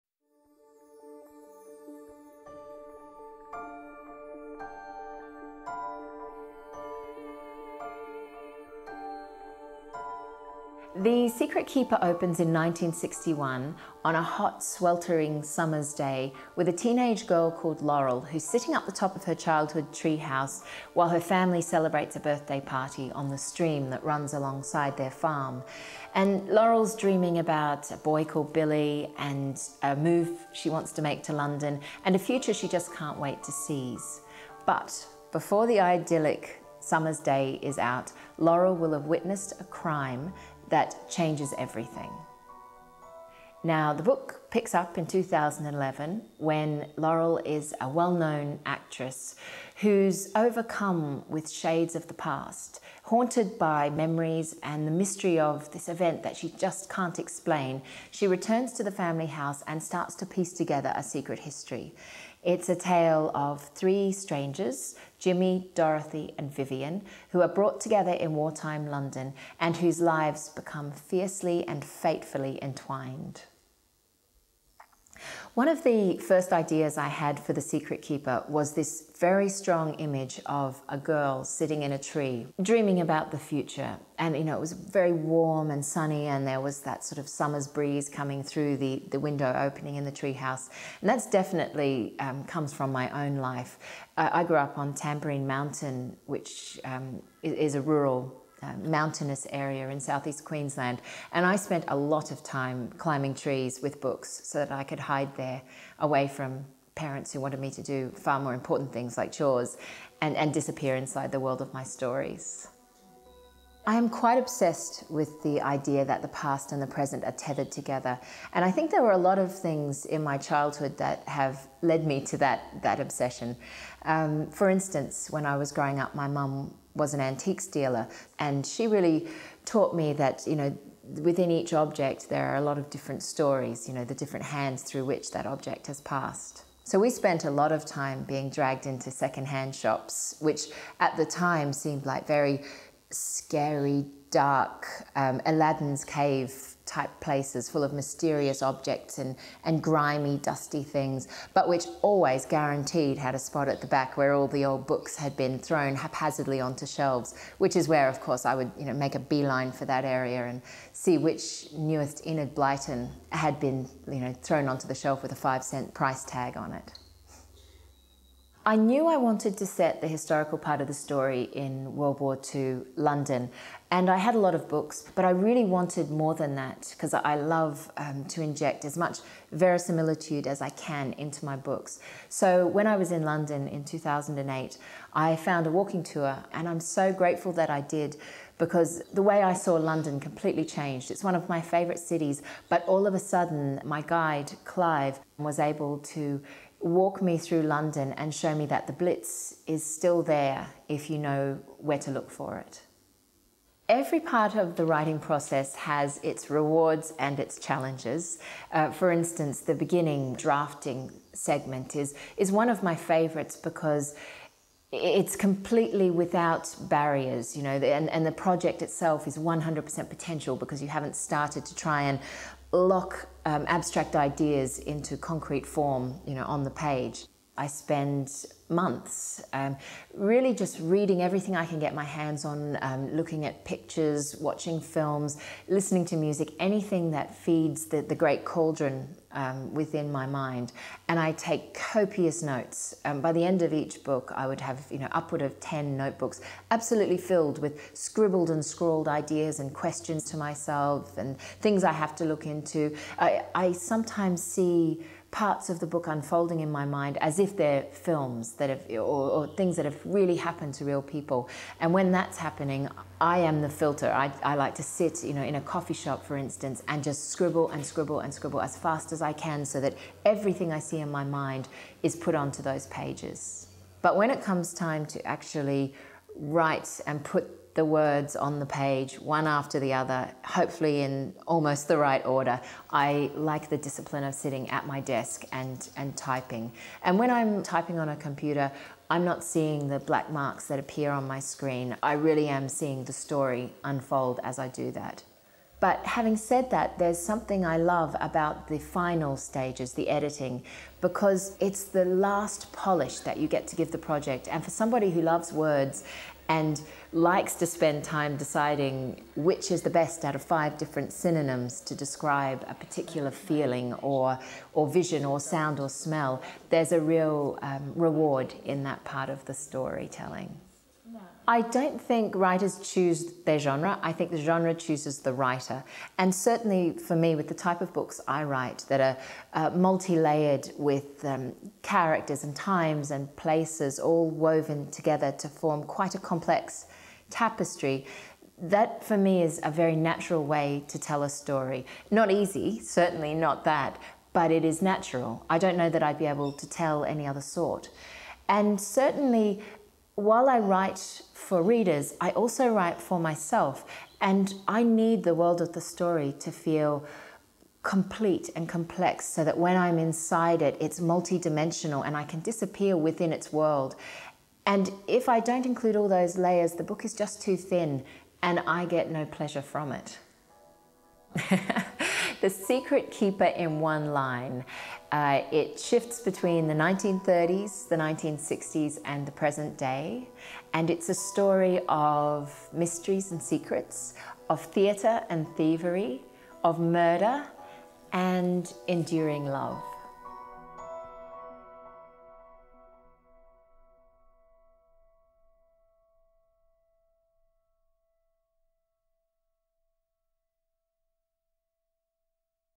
The Secret Keeper Kate Morton
Kate Morton introduces her fourth novel, The Secret Keeper, and discusses some of the ideas and inspiration behind its creation